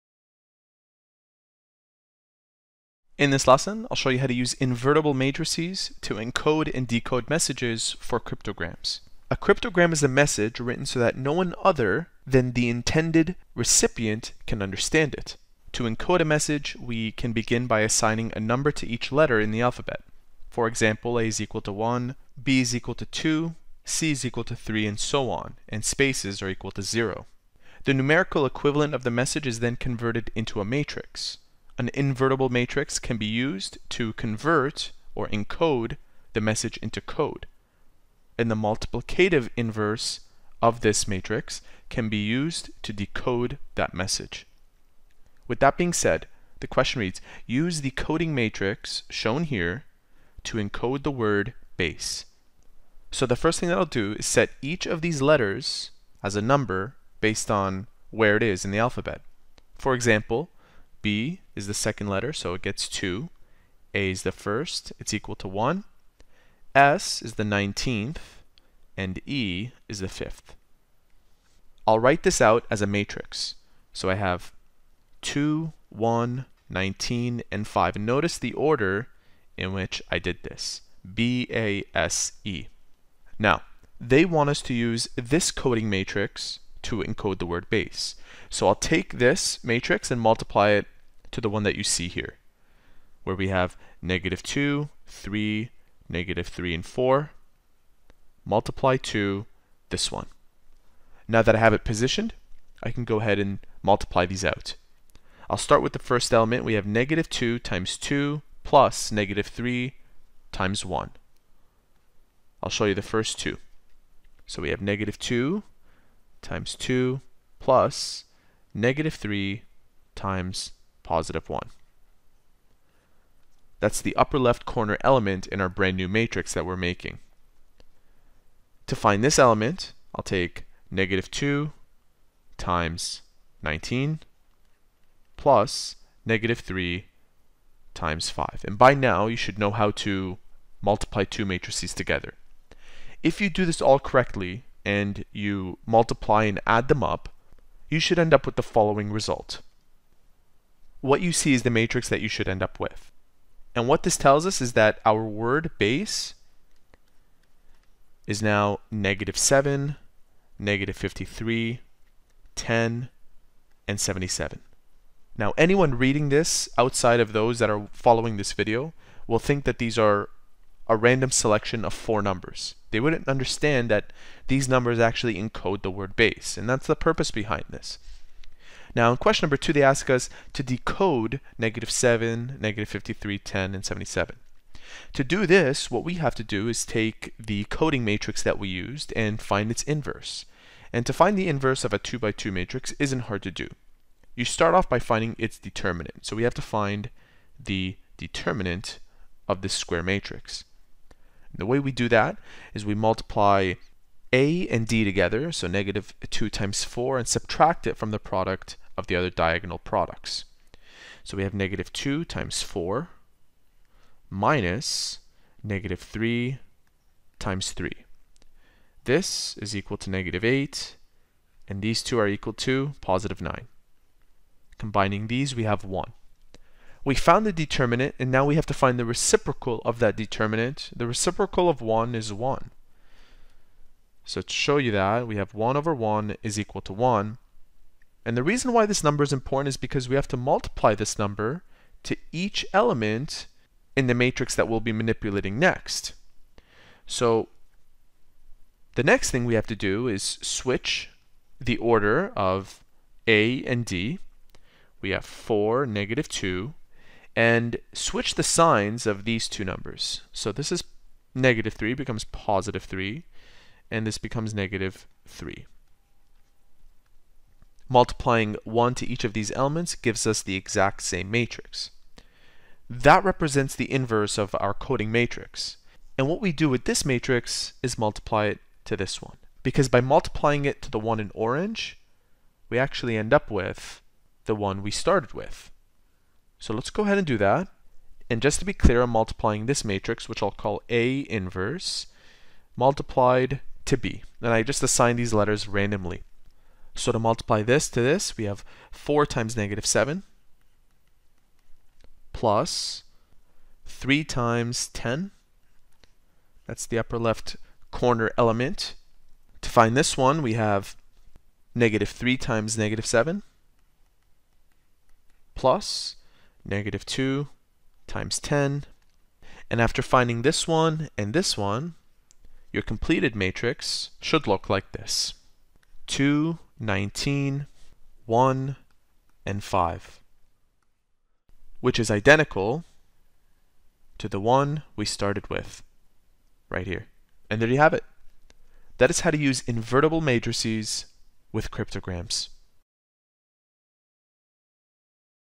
📚 Using invertible matrices with cryptograms
A cryptogram is a message written so that no one other than the intended recipient can understand it. To encode a message, we can begin by assigning a number to each letter in the alphabet:

A = 1, B = 2, C = 3, … Z = 26, and spaces = 0.

The numerical equivalent of the message is then converted into a matrix. An invertible matrix can be used to convert (encode) the message into code. The multiplicative inverse of this matrix can be used to decode the message.

Q1.   Use the coding matrix ... to encode the word BASE.

Q2.   Decode −7,   10,   −53,   77.

* Encoding a Word or Message

1) Express the word or message numerically.

2) List the numbers in step 1 by columns and form a square matrix. If you do not have enough numbers to form a square matrix, put zeros in any remaining spaces in the last column.

3) Select any square invertible matrix, called the coding matrix, the same size as the matrix in step 2. Multiply the coding matrix by the square matrix that expresses the message numerically. The resulting matrix is the coded matrix.

4) Use the numbers, by columns, from the coded matrix in step 3 to write the encoded message.

* Decoding a Word or Message That Was Encoded

1) Find the multiplicative inverse of the coding matrix.

2) Multiply the multiplicative inverse of the coding matrix and the coded matrix.

3) Express the numbers, by columns, from the matrix in step 2 as letters.